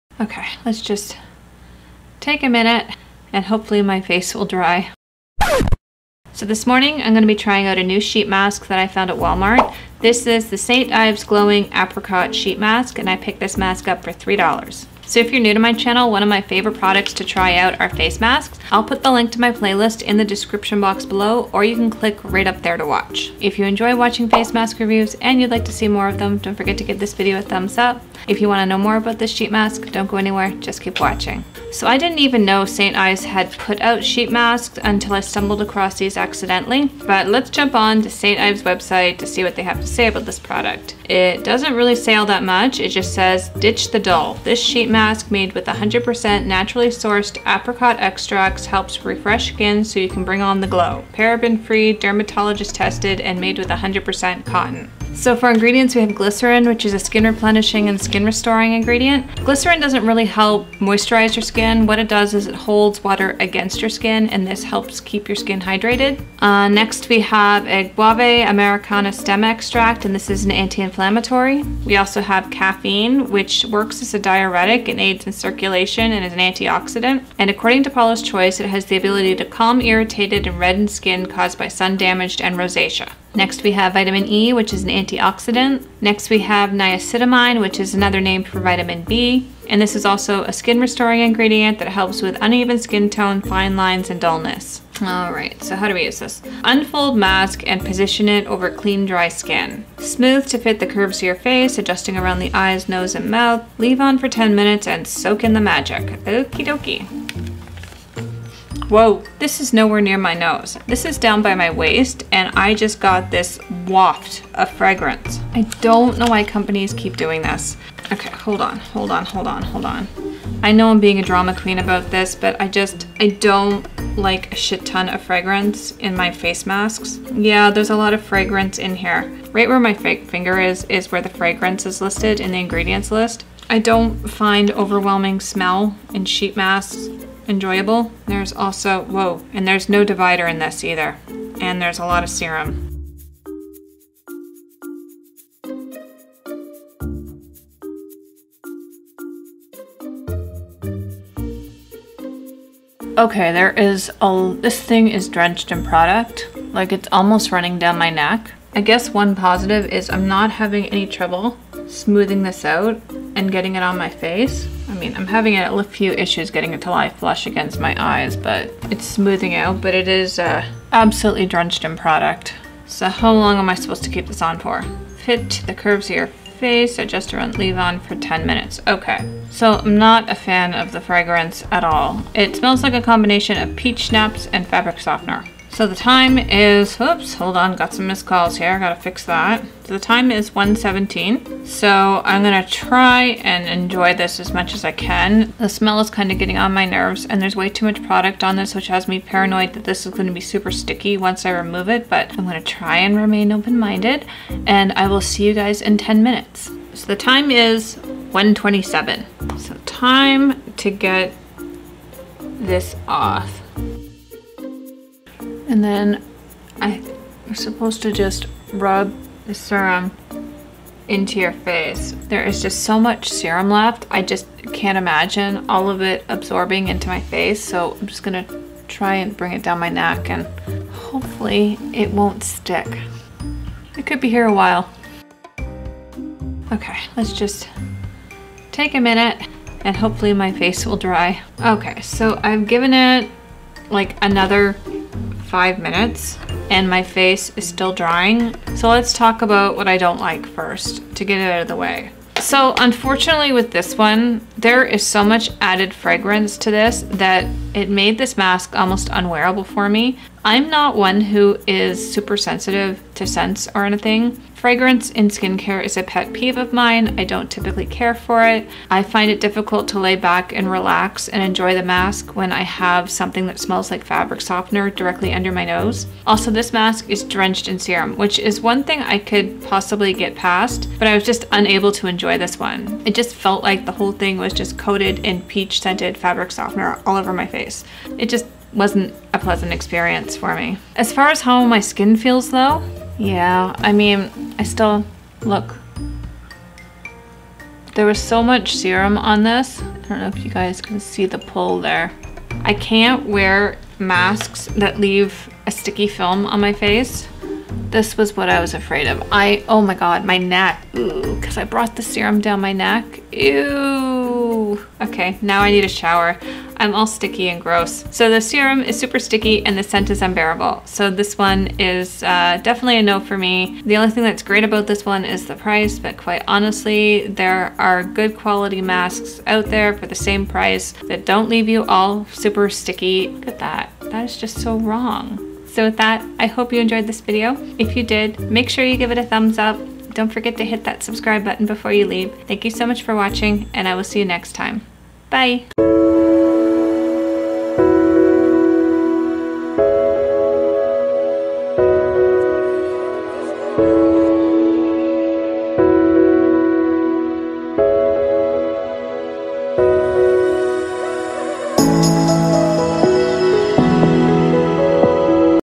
St Ives Glowing Apricot Sheet Mask
St  Ives Glowing Apricot Sheet Mask | Mask Monday

Hi All! Join me as I test out this new face mask from St. Ives, the Glowing Apricot sheet mask.  I love face masks, they are some of my favorite pamper products for my over 40 dry skin. This drugstore sheet mask claims to helps refresh skin leaving it glowing & made with 100% naturally sourced apricot extract.  What are some of your favorite face masks?

❋  Hautelook (Nordstrom Rack flash sale site- makeup, shoes, etc)

▼▼▼ SOCIAL MEDIA ▼▼▼

I use MagicLinks for all my ready-to-shop product links.

PRODUCTS I MENTIONED:
* St. Ives Apricot Glowing Sheet Mask